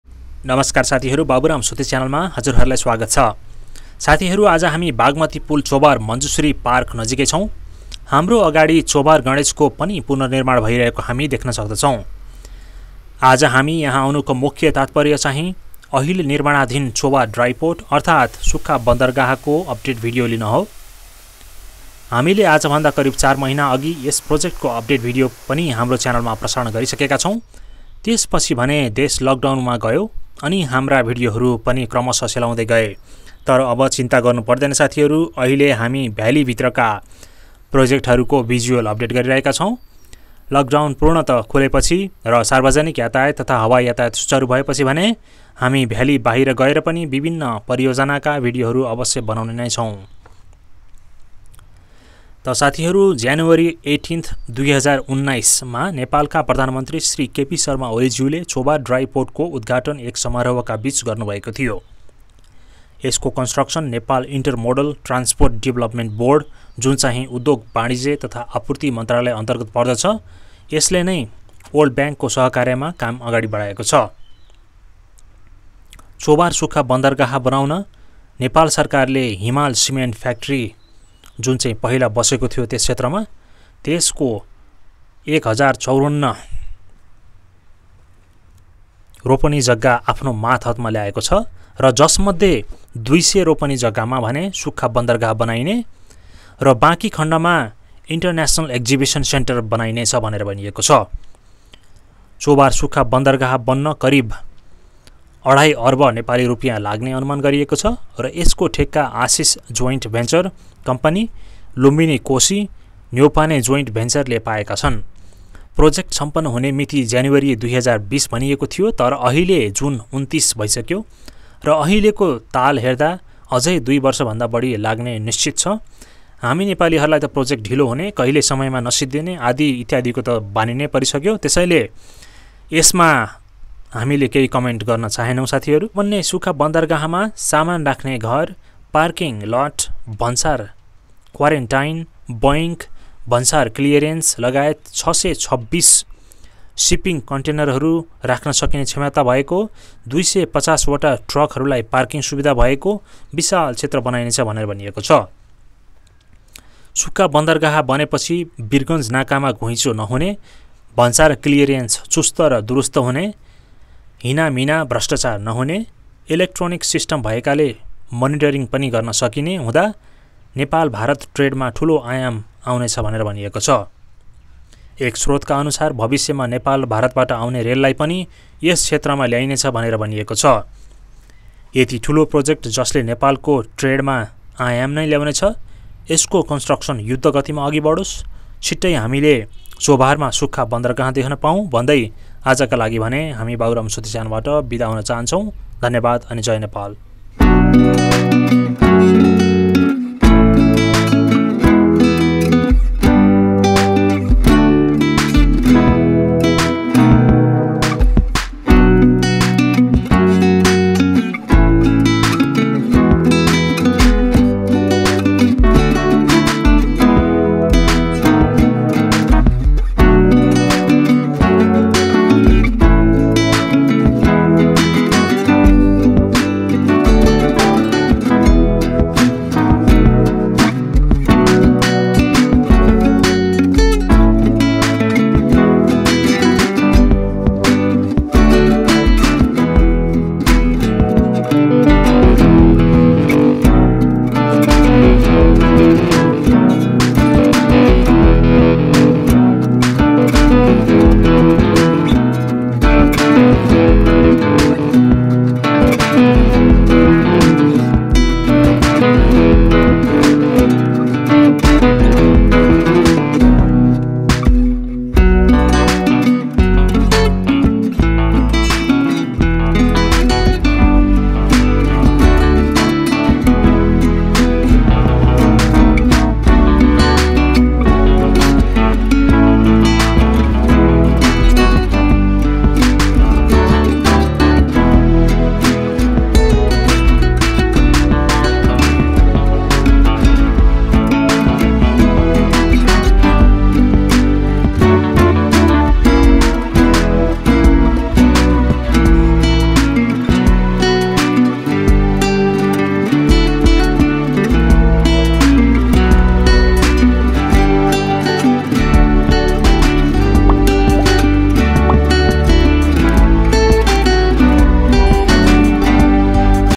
Chobhar Dry Port Construction Update. चोभारमा यसरी निर्माण हुँदै छ सुख्खा बन्दरगाहा ॥
The latest development on ongoing construction of Chobhar Dry Port at Chobhar of Kathmandu Nepal has been showcased in this video.
      We have been following this project for about a year and a half now and that we shall be following until the end of the completion.
This video is the property of Baburamsoti Channel.